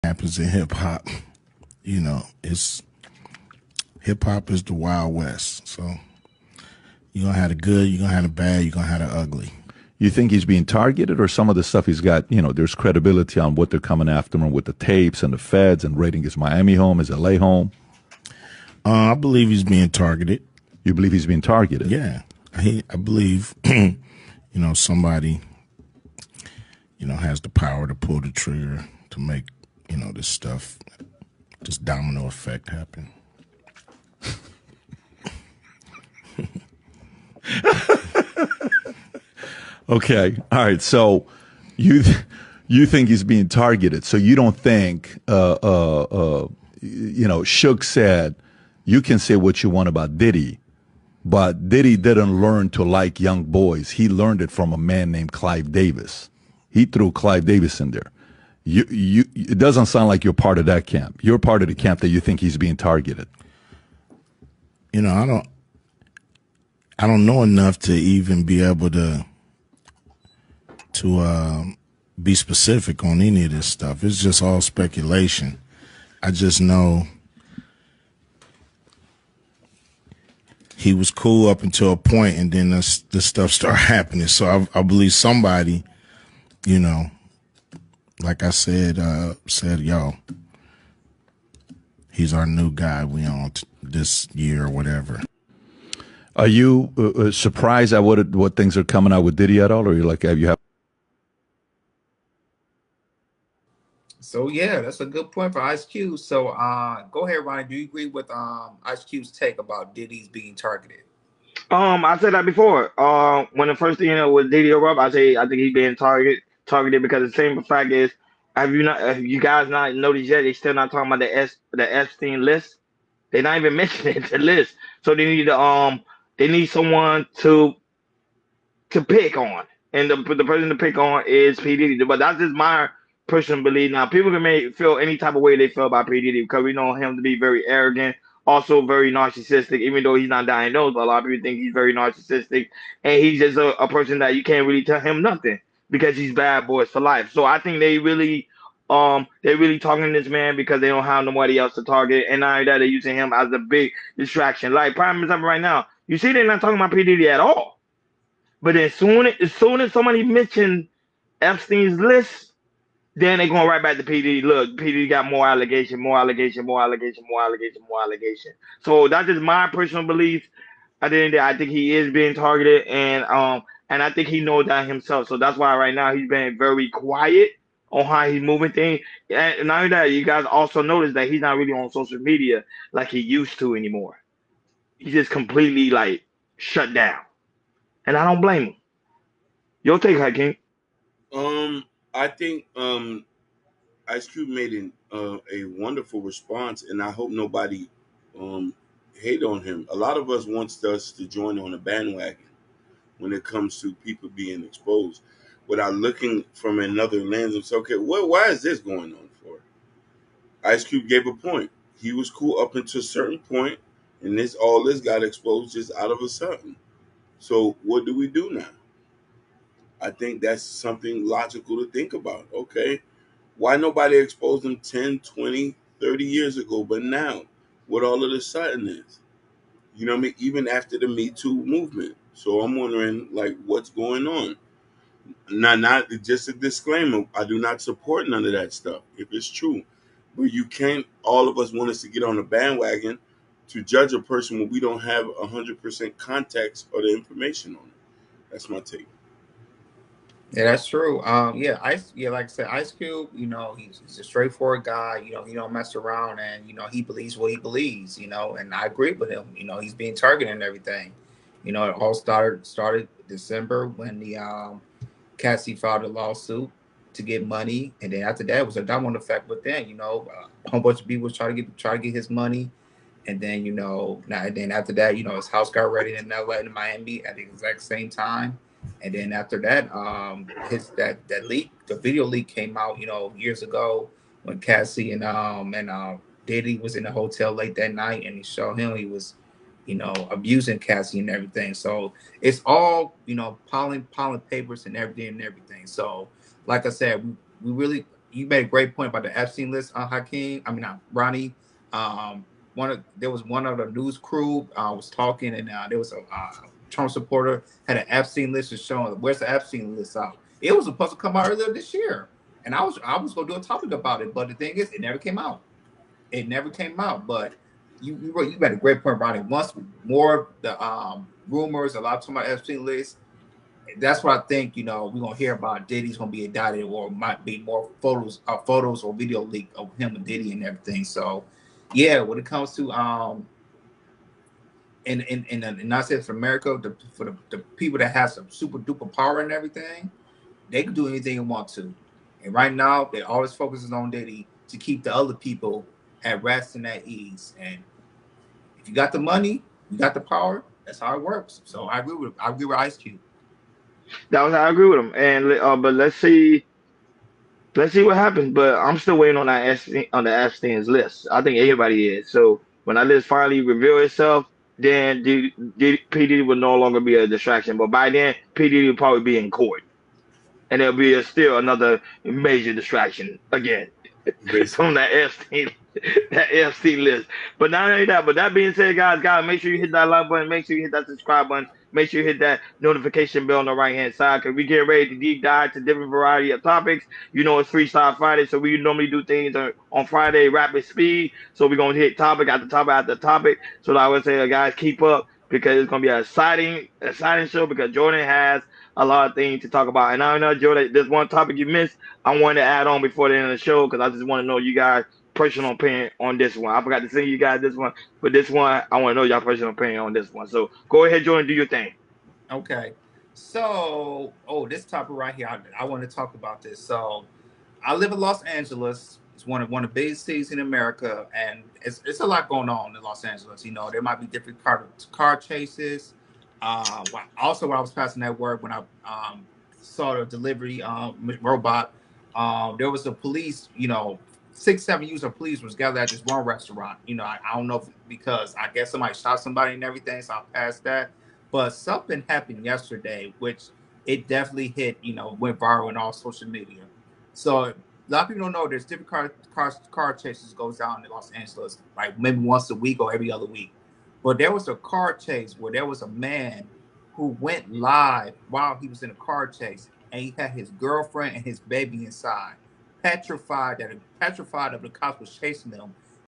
Ice Cube Believes Sean P. Diddy Is Being Targeted By The Media
Ice Cube Believes Sean P. Diddy Is Being Targeted By The Media ​⁠icecube